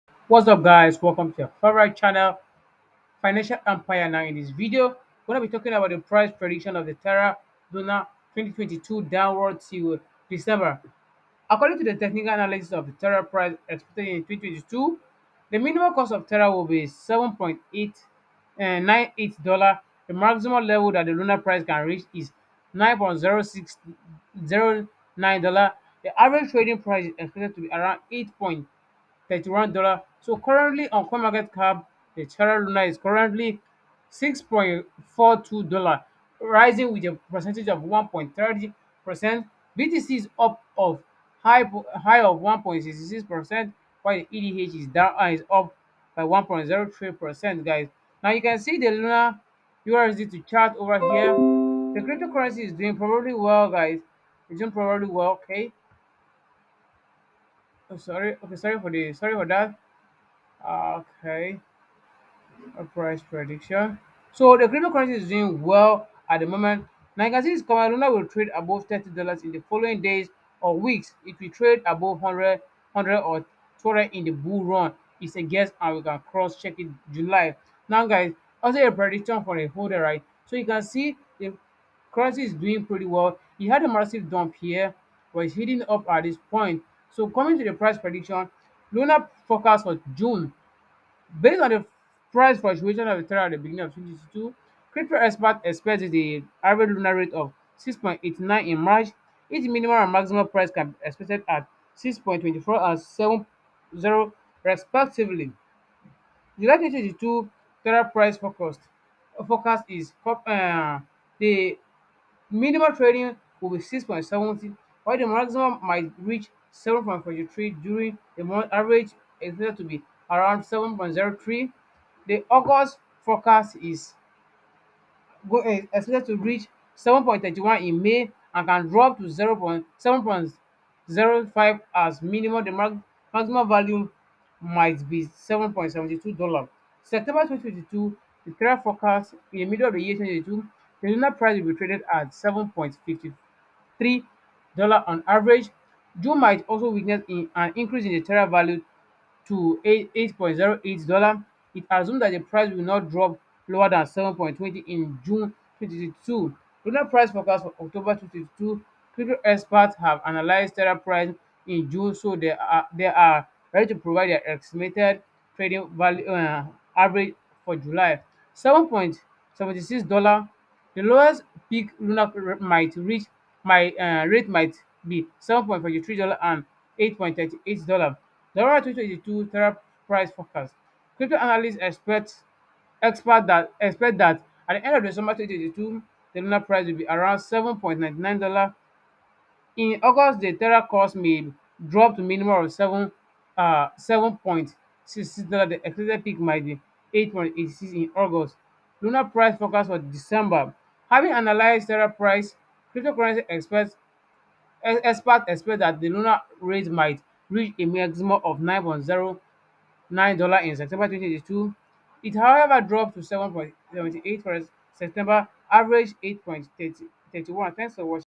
Terra Luna Price Prediction 2022 - 2031
In this video I am going to talk about the terra luna price prediction. We will be discussing terra luna,terra luna crypto and luna classic news .